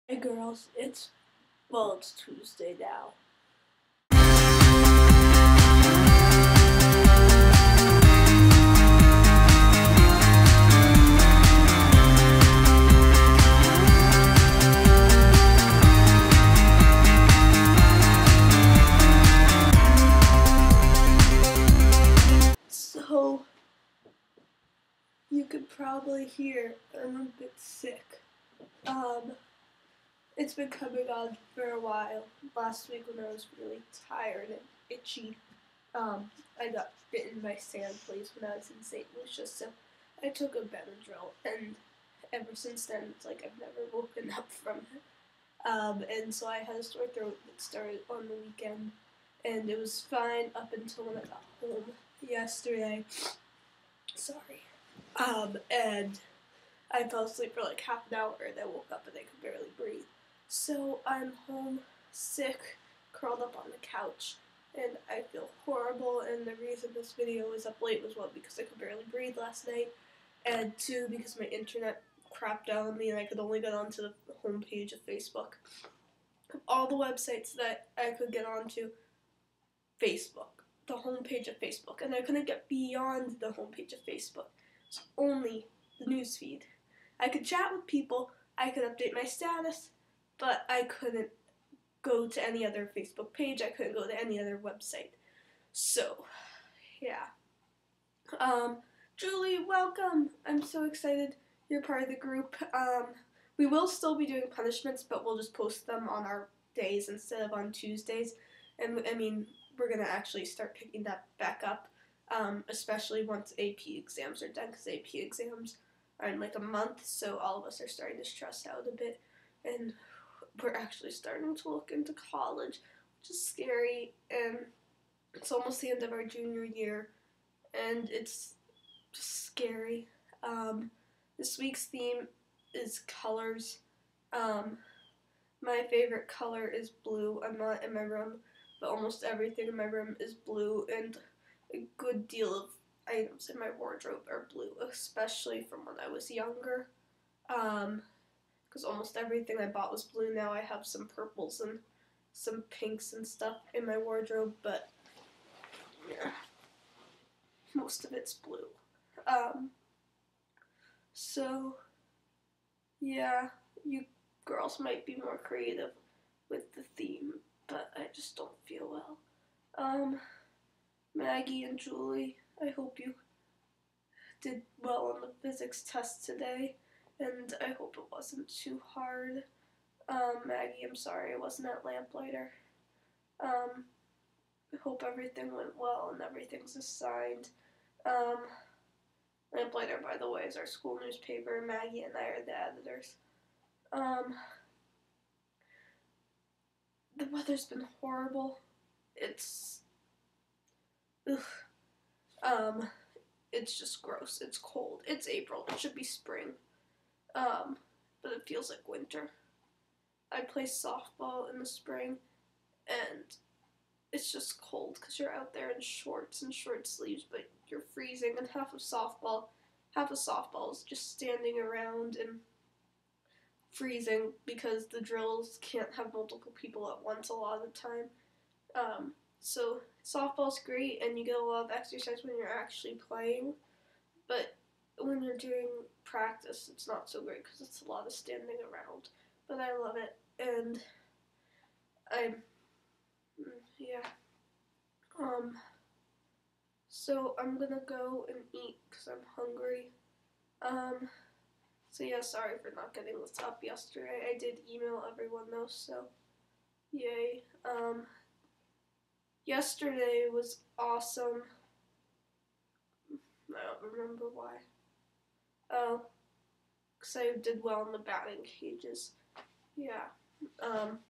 In Which Rachel is Sick
I wasn't gonna make a video this week, but I'm home sick with nothing else to do...